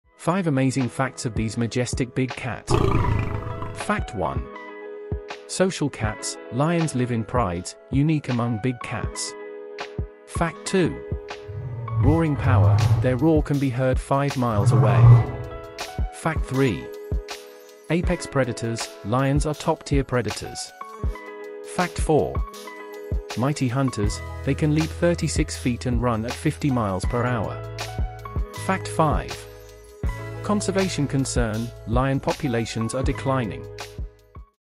5 Amazing Lions 🐅🐅👑️
Lions are the kings of the jungle, but there's more to them than just their mane and roar. Here are 5 amazing facts about lions that you probably didn't know.